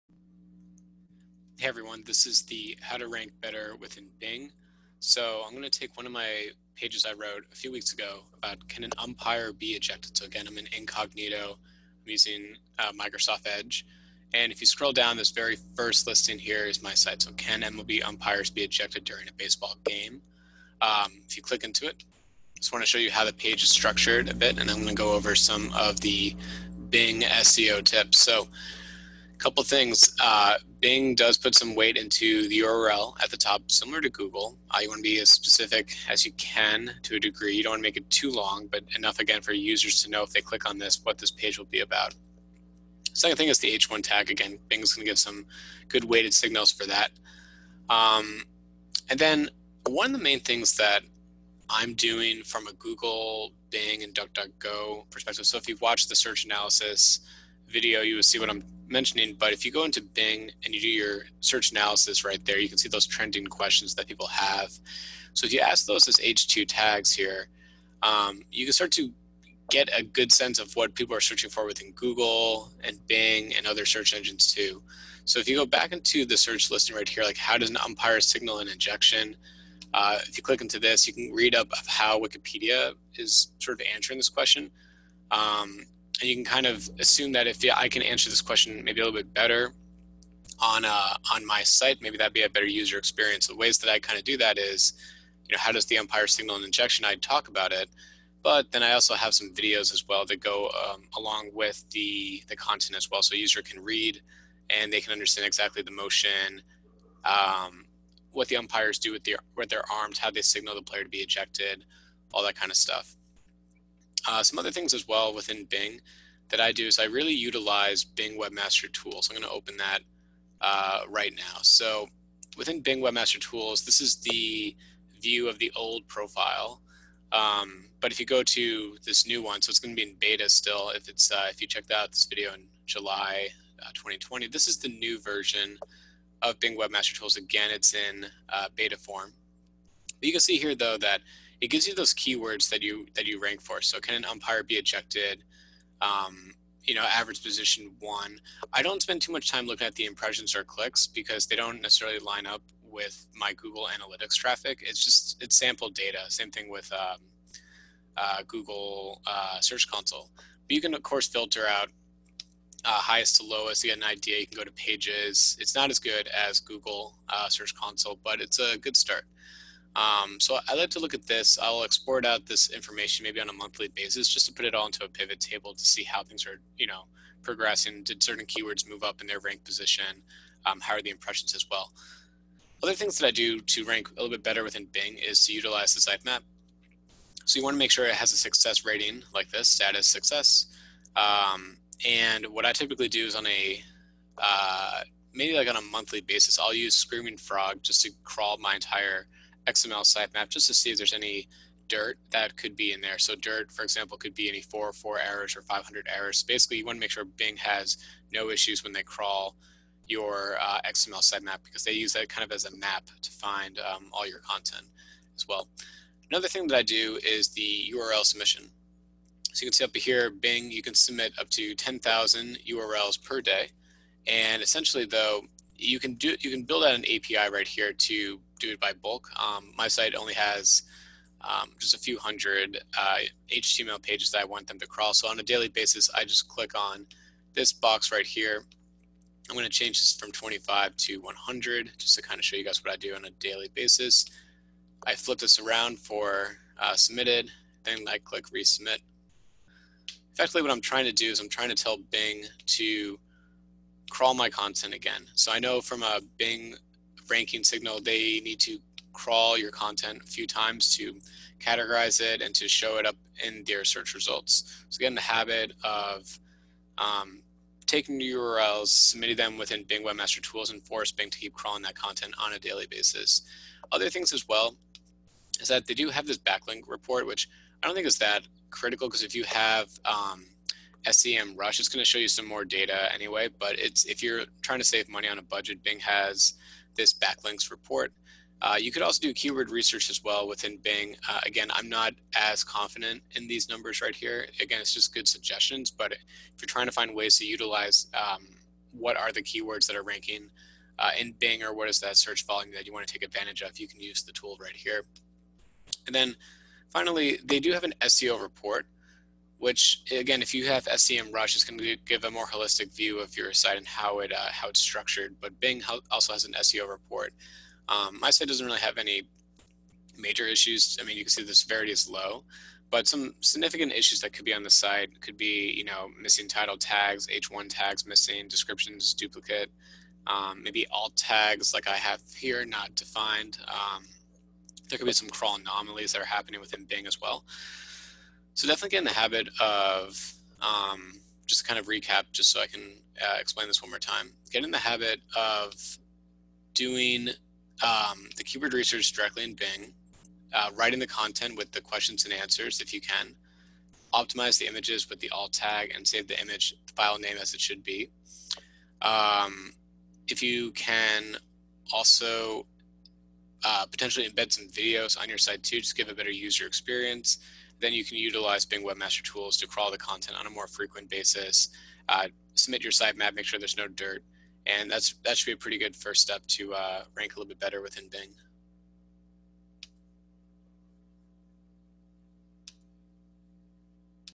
How to Rank Better in Microsoft Bing - SEO Workshop
Microsoft Bing can deliver extra traffic to your site if you optimize for it in a few ways. Just like Google, Bing looks for high-quality content, domain authority, internal links, external links, and more. Here are my additional tips on how you can use tools like Bing Webmaster Tools to improve your presence in Microsoft Bing